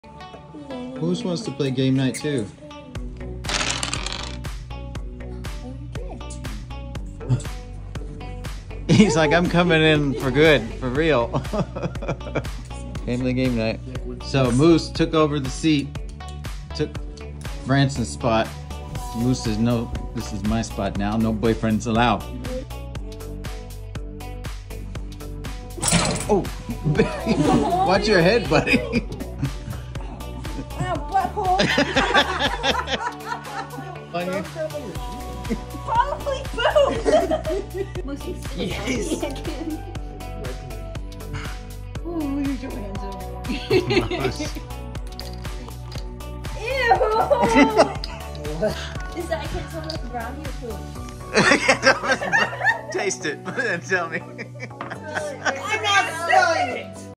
Family Game Night w/ Great Dane MinnesotaMoose!
When Moose feels left out and wants attention, he’ll be sure to let you know. 😂